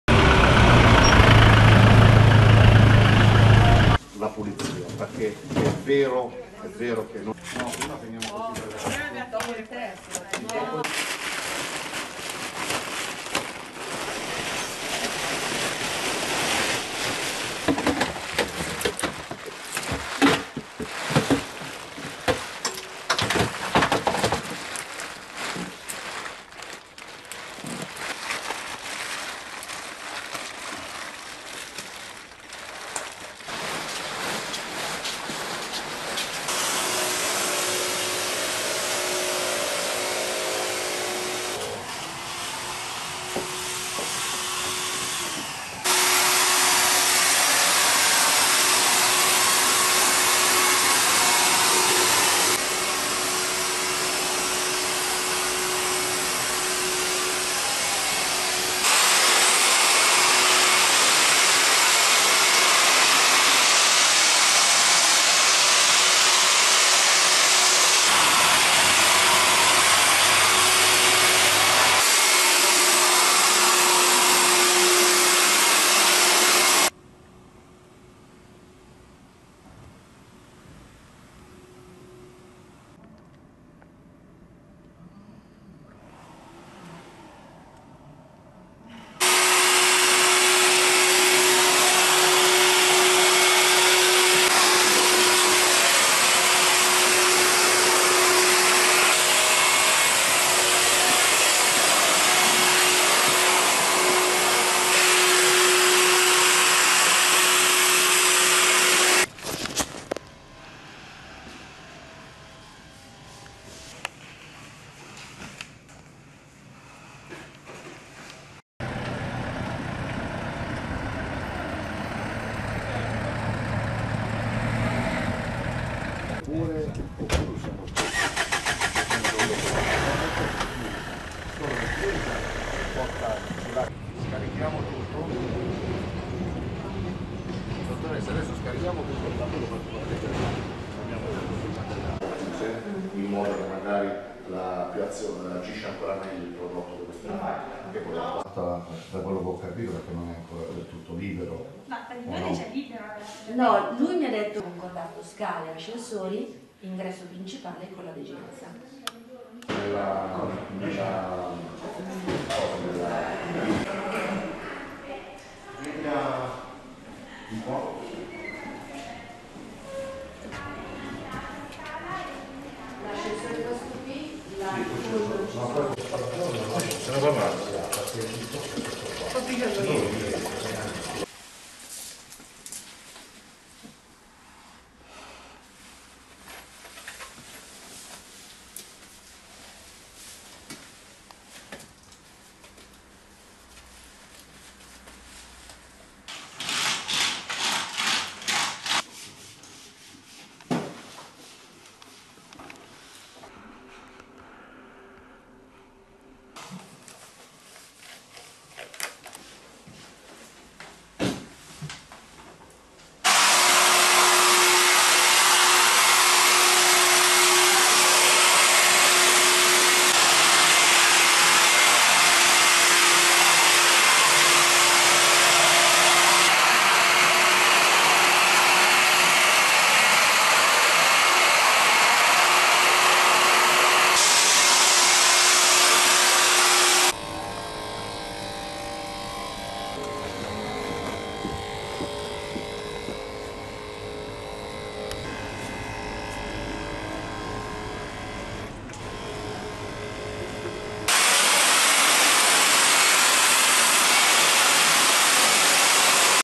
Coronavirus, intervento della Nato per la disinfezione degli ospedali lombardi
Nell’ambito della Fase 2 della risposta alla pandemia di coronavirus, il Corpo di Reazione rapida della NATO di stanza a Solbiate Olona, a nord di Milano, sta conducendo e coordinando una serie di attività a sostegno delle comunità e delle autorità locali. Lo comunica in una nota il quartier generale dell’Alleanza Atlantica a Bruxelles, sottolineando che gli interventi delle forze armate comprendono il dispiegamento di un team di disinfezione negli ospedali civili locali, per mantenere le strutture chiave sicure e sanificate per i pazienti, così come l’approvvigionamento di forniture mediche critiche (quali tute, ventilatori, maschere, guanti e disinfettante) e il supporto logistico agli operatori sanitari in prima linea nella lotta contro il Coronavirus. Il Corpo di Reazione Rapida NATO in Italia è una realtà generalmente utilizzata per pianificare ed eseguire operazioni militari ad alta intensità, destinate al contrasto delle crisi e la promozione della stabilità internazionale. Grazie all’eccellenza nell’innovazione e all’alta adattabilità, questo comando multinazionale ha operato in varie zone di conflitto, come l’Afghanistan, ma è anche attivo in varie emergenze civili, quali, appunto, la crisi legata all’epidemia di Coronavirus. (Video NATO Rapid Deployable Corps Italy)